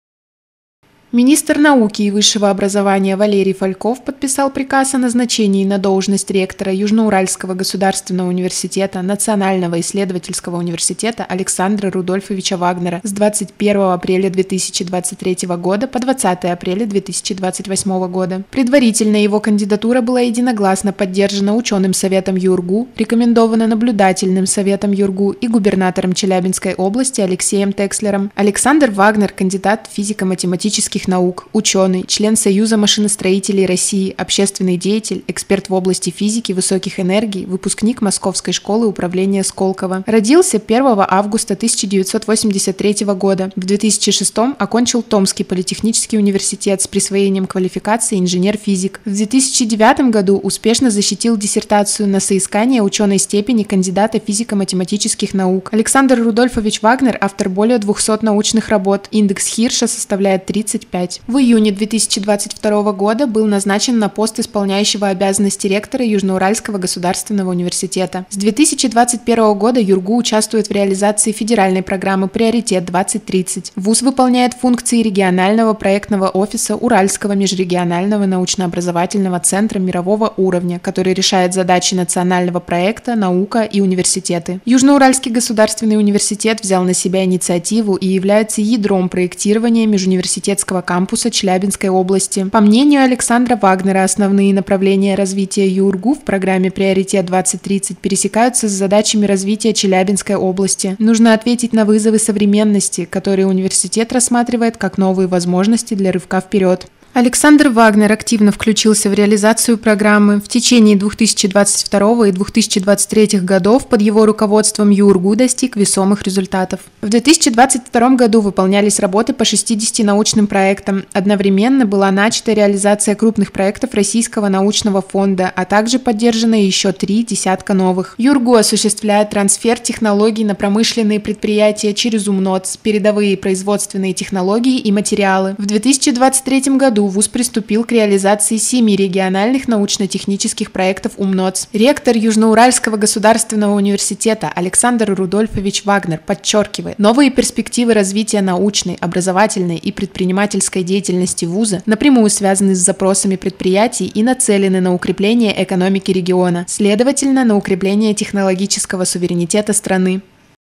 Александр Вагнер - ректор
Александр Рудольфович Вагнер утвержден в должности ректора Южно-Уральского Государственного университета.